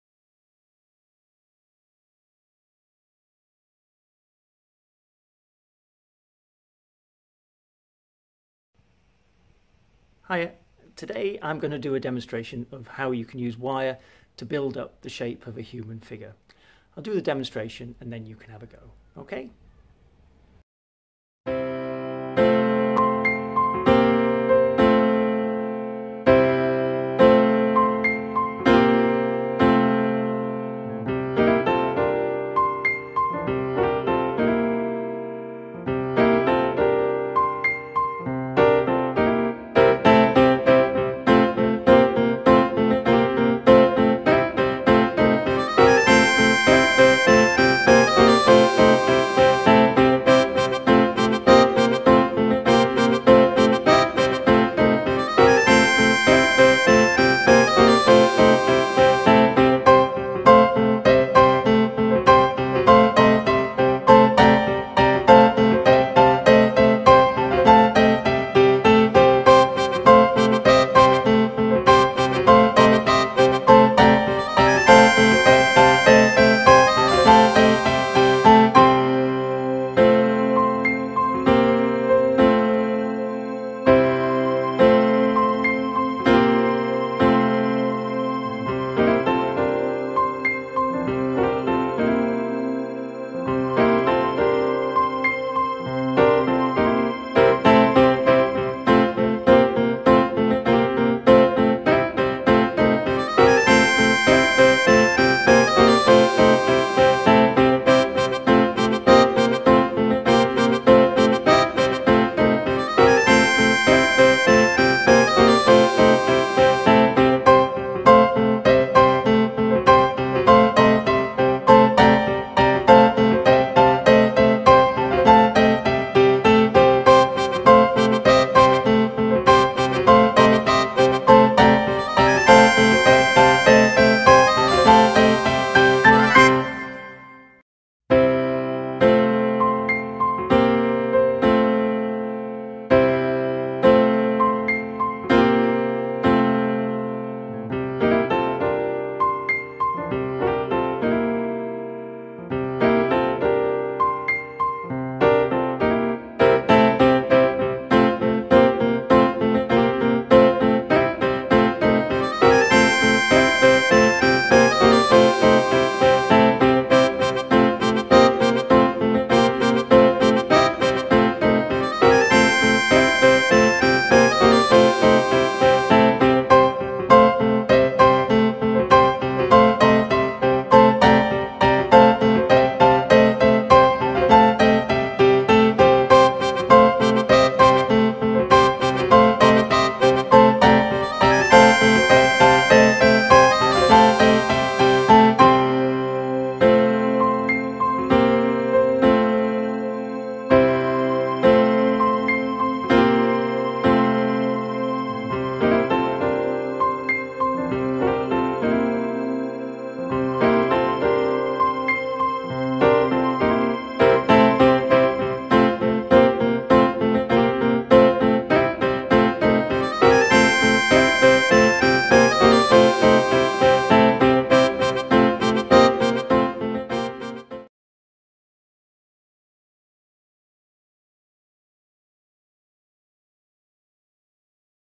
Wire sculpting
A demonstration of how to make a figure out of wire. Art teachers and students, get your copy of The Complete Art Curriculum